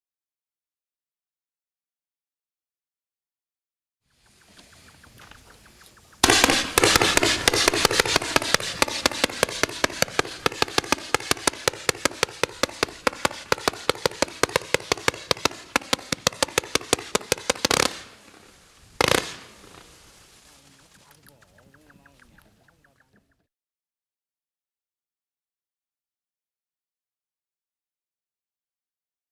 Röder Feuerwerk Sternenstaub, Neuheit 2016
Sternenstaub ist Neuheit von Röder Feuerwerk und ist in Zusammenarbeit mit Fireevent entstanden. Eine Fächerbatterie mit blinkenden Goldschweifkometen mit bunten Spitzen in rot, blau und grün.

Muster-Video aus China.